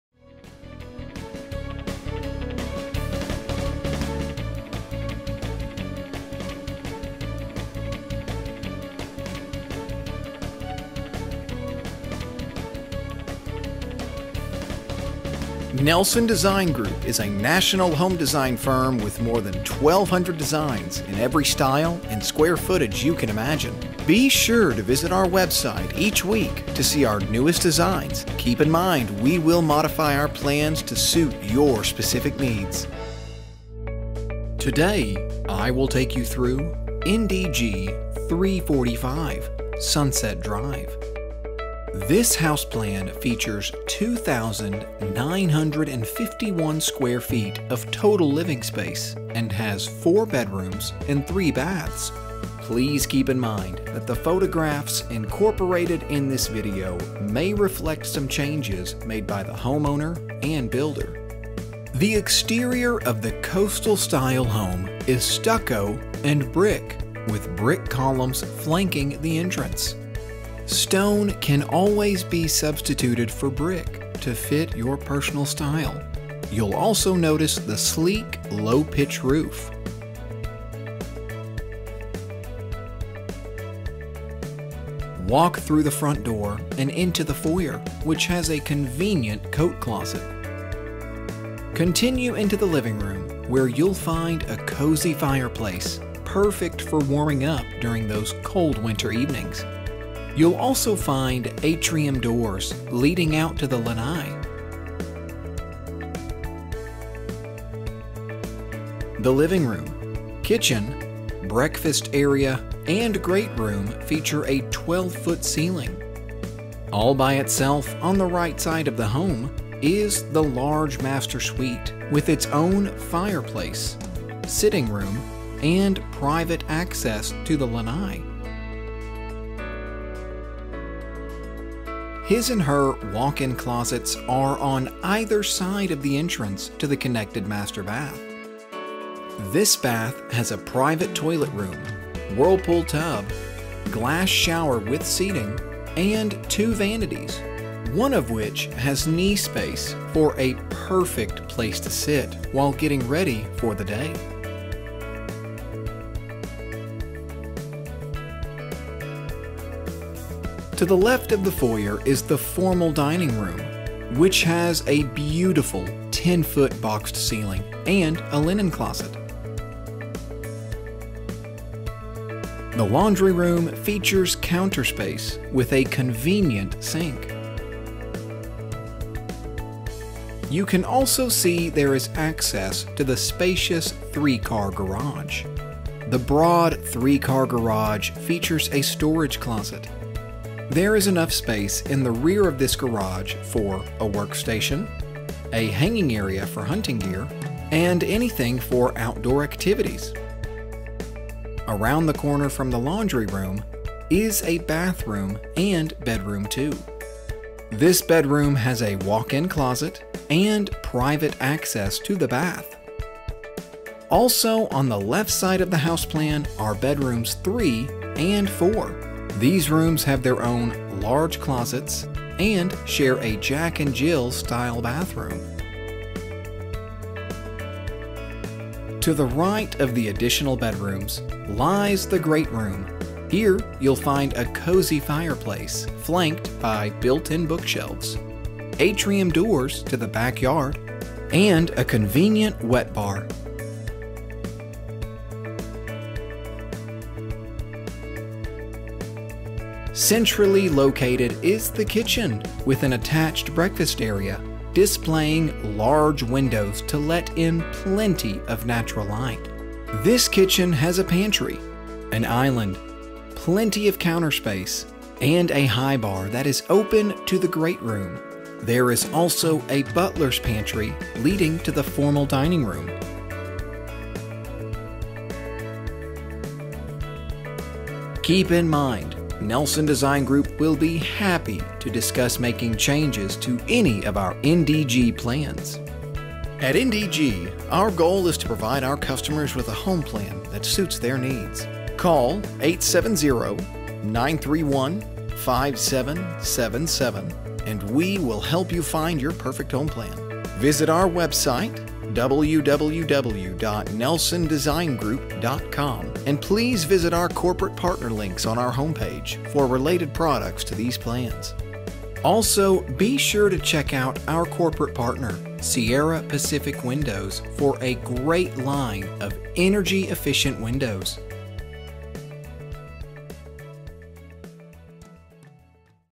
House Plan NDG 345 (Sunset Drive) Visual Open House
NDG 345 - Sunset Drive
2,951 Sq.Ft. | 4 Bed | 3 Bath

This sprawling four-bedroom plan centers on an ultra functional kitchen accessing all living areas with ease. A handy eat-at bar and island help make this kitchen any gourmet’s dream. Separate living and great rooms each feature gas fireplaces and lots of open space. All three secondary bedrooms feature private bath entrances. The exquisite master suite presents a radiant fireplace, large sitting area, private entrance to the lanai and a massive bath that includes his and her walk-in closets and an oversized whirlpool tub.

Photos courtesy of Joe Wayman Homes, IN; brick exterior per builder

and our Corporate Partner links on our homepage for related products that compliment these plans.